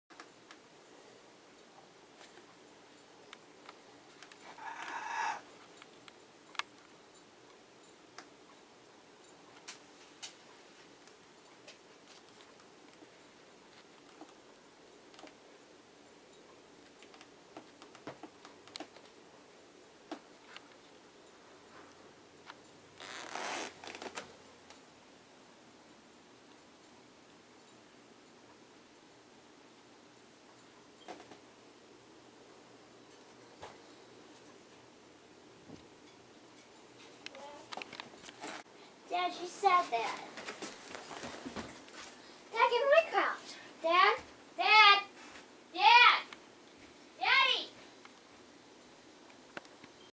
Lazy silly cats in HD
Katie and Jo Jo doing what they do best!